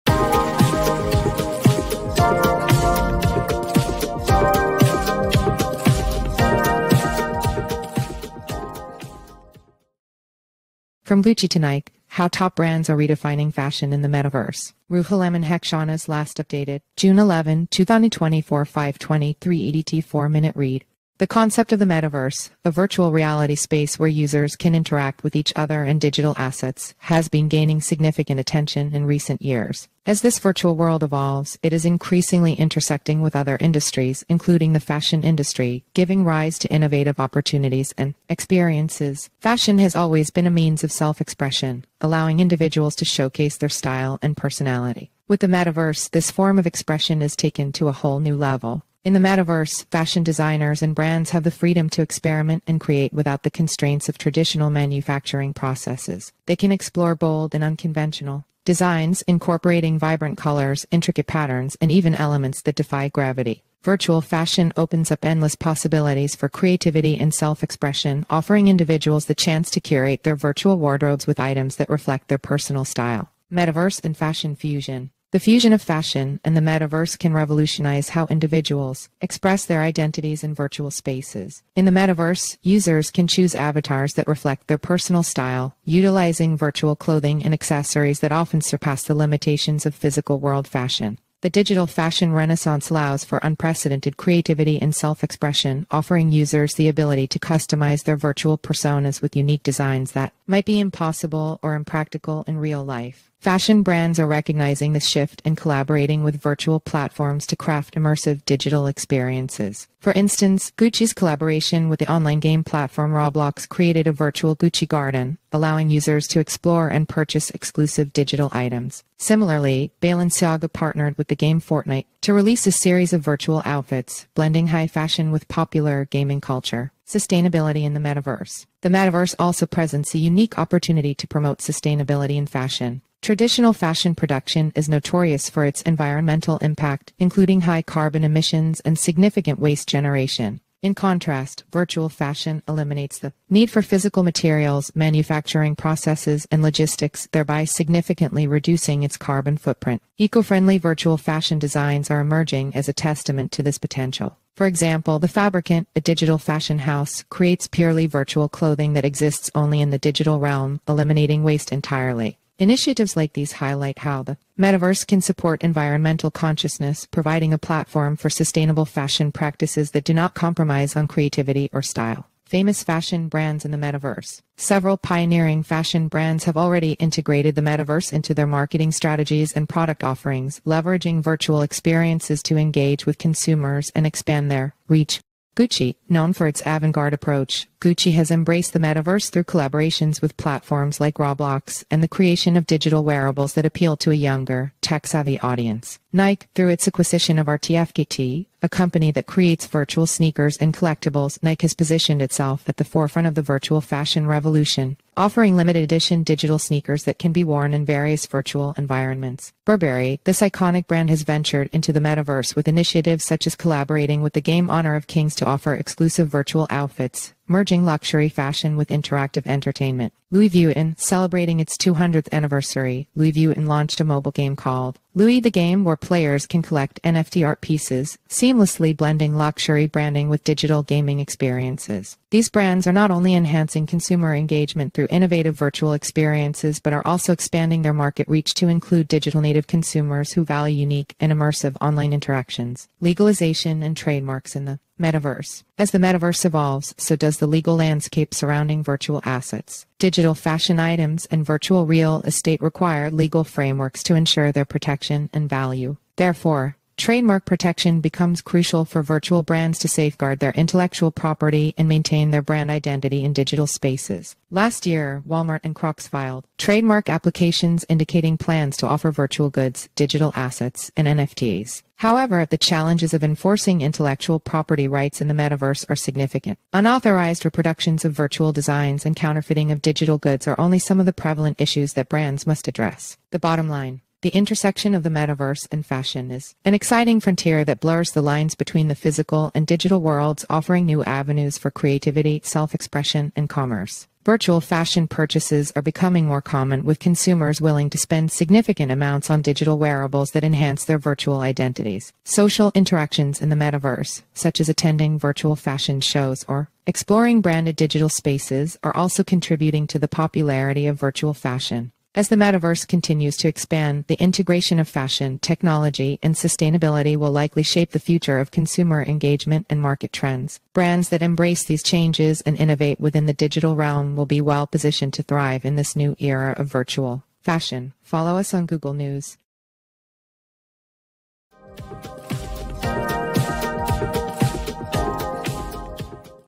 From Gucci to Nike: How Top Brands are Redefining Fashion in the
Ruholamin Haqshanas

Last updated:

June 11, 2024 05:23 EDT

The concept of the metaverse, a virtual reality space where users can interact with each other and digital assets, has been gaining significant attention in recent years.

As this virtual world evolves, it is increasingly intersecting with other industries, including the fashion industry, giving rise to innovative opportunities and experiences.

Fashion has always been a means of self-expression, allowing individuals to showcase their style and personality. With the metaverse, this form of expression is taken to a whole new level.

In the metaverse, fashion designers and brands have the freedom to experiment and create without the constraints of traditional manufacturing processes.

They can explore bold and unconventional designs, incorporating vibrant colors, intricate patterns, and even elements that defy gravity.

Virtual fashion opens up endless possibilities for creativity and self-expression, offering individuals the chance to curate their virtual wardrobes with items that reflect their personal style.

Metaverse and Fashion Fusion.

The fusion of fashion and the metaverse can revolutionize how individuals express their identities in virtual spaces.

In the metaverse, users can choose avatars that reflect their personal style, utilizing virtual clothing and accessories that often surpass the limitations of physical world fashion.

The digital fashion renaissance allows for unprecedented creativity and self-expression, offering users the ability to customize their virtual personas with unique designs that might be impossible or impractical in real life.

Fashion brands are recognizing this shift and collaborating with virtual platforms to craft immersive digital experiences.

For instance, Gucci’s collaboration with the online game platform Roblox created a virtual Gucci Garden, allowing users to explore and purchase exclusive digital items.

Similarly, Balenciaga partnered with the game Fortnite to release a series of virtual outfits, blending high fashion with popular gaming culture.

Sustainability in the Metaverse.

The metaverse also presents a unique opportunity to promote sustainability in fashion.

Traditional fashion production is notorious for its environmental impact, including...